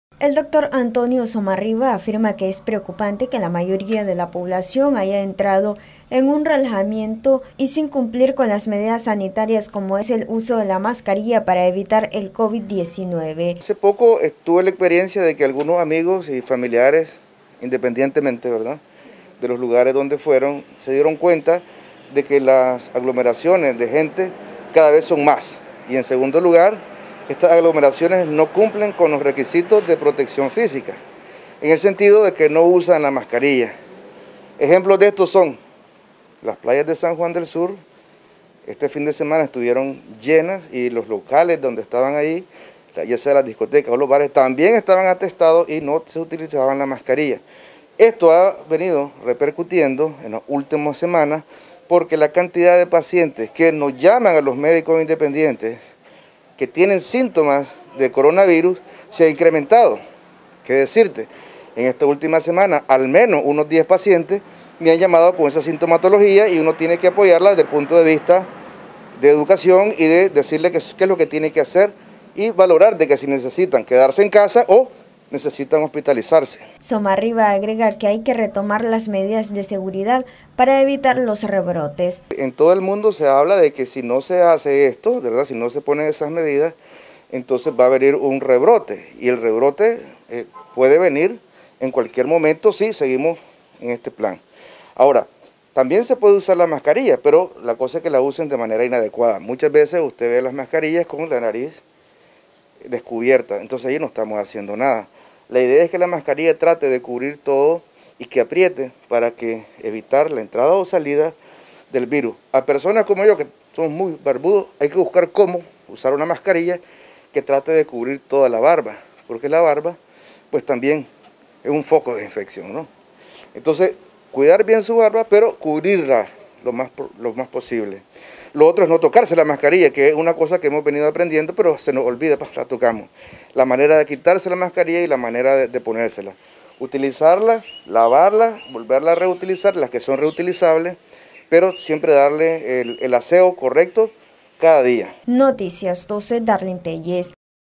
La población no debe bajar la guardia ante la pandemia del covid-19.
NosResistimosAlEmbargo El especialista Antonio Somarriba, manifestó que es preocupante que parte de la población este participando en actividades masivas sin el debido uso de la mascarilla.